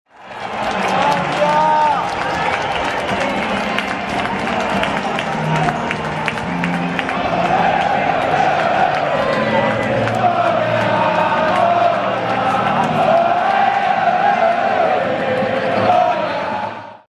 Sampdoria-Atalanta 2019/2020
Sampdoria-Atalanta 10 novembre 2019, cori e coreografia ad inizio partita.

Credits: LUCADEA.com